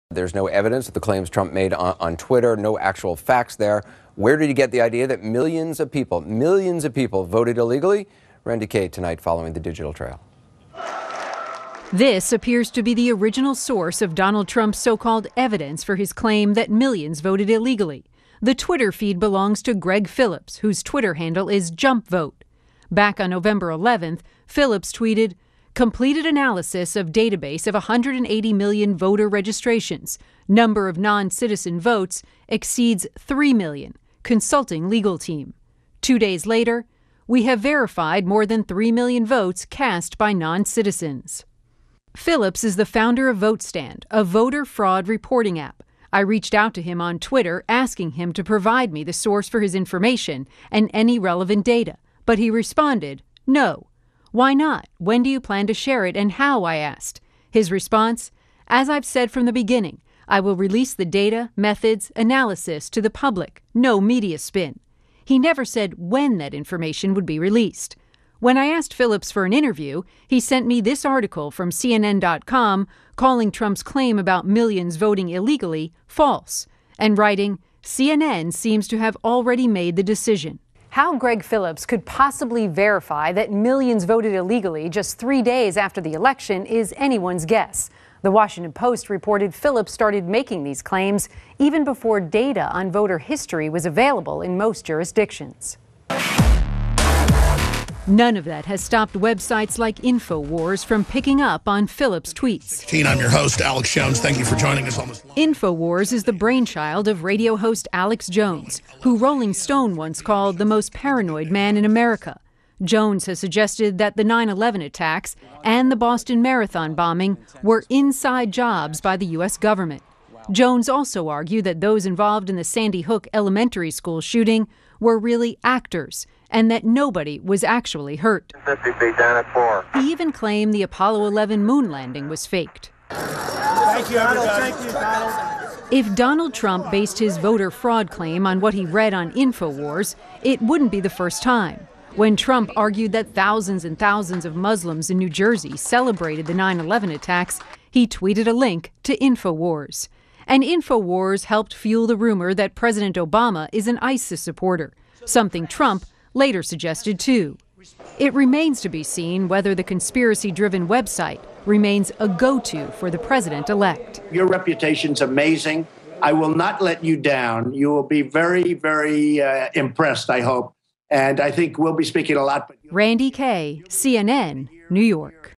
11-29-2016 Tracing Trump’s false claims that millions voted illegally (from shady InfoWars site?)
It sounds like president elect ‘Crazy Ass’ is getting his information from the shady conspiracy website ‘InfoWars’.  I actually know people who believe websites like that.